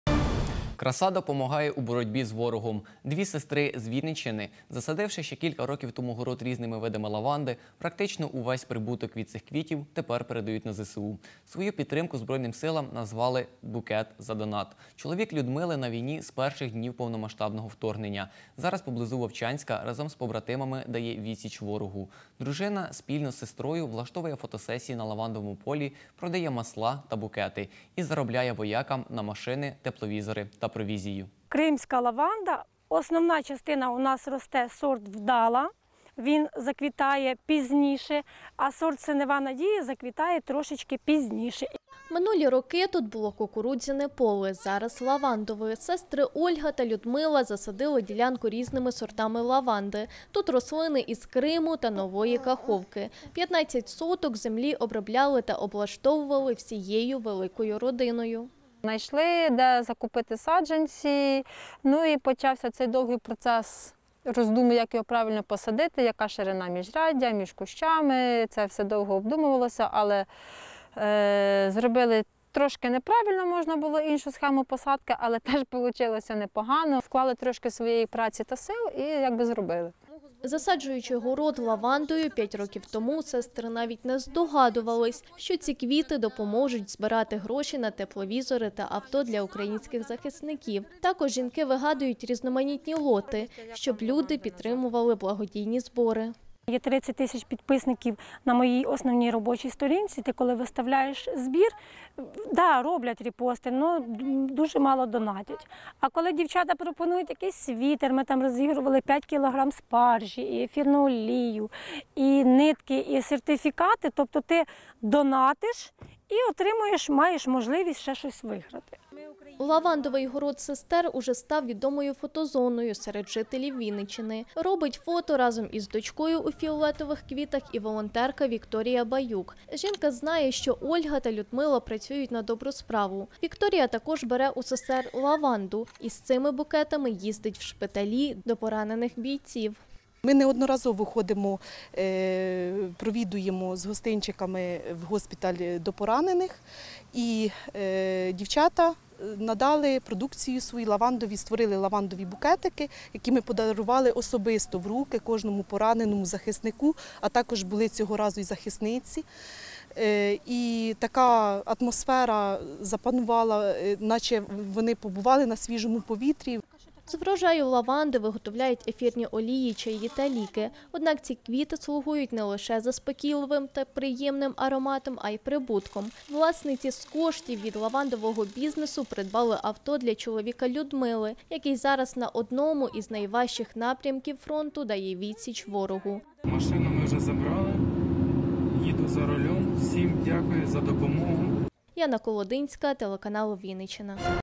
Лаванда на ЗСУ
Краса, допомагає у боротьбі з ворогом. Дві сестри  з Вінниччини засадивши ще кілька років тому город різними видами лаванди, практично увесь прибуток від цих квітів  тепер передають на ЗСУ. Чоловік Людмили на війні з перших днів повномасштабного вторгнення, зараз поблизу Вовчанська разом з побратимами дає відсіч ворогу. Дружина спільно з  сестрою влаштовує фотосесії на лавандовому полі, продає масла та букети й заробляє воякам на машини, тепловізори та провізію.